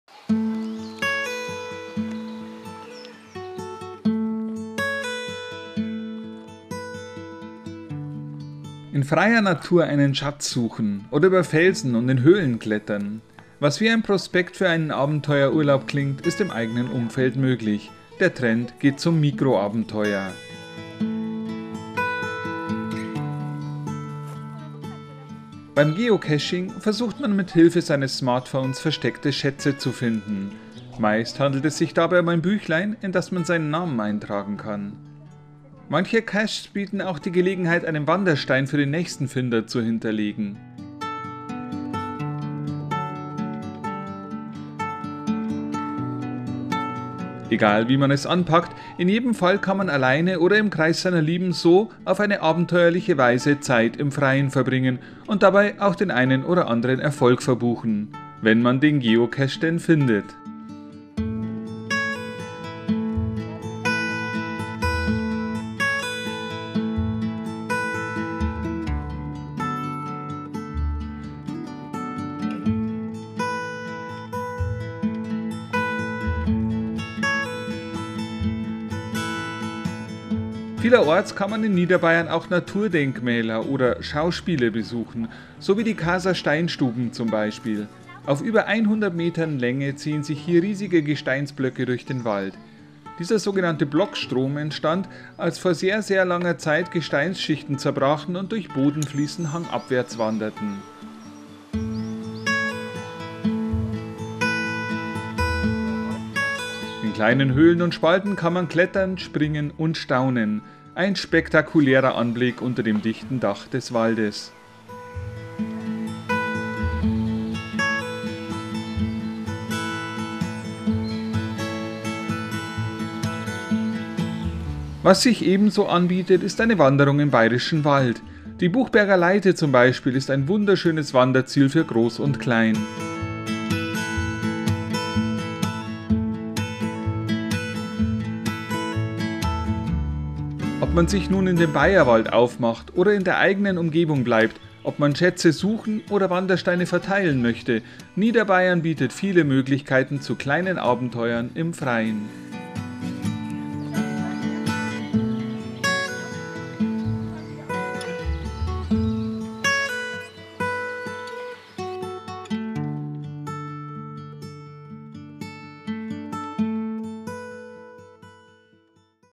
Ferientipps für Familien: Mikroabenteuer vor der eigenen Haustür
Unser Redakteur Andreas Reichelt sammelt für Sie Tipps für die Ferien. Heute hat er sich mit kleinen Abenteuern in näherer Umgebung beschäftigt.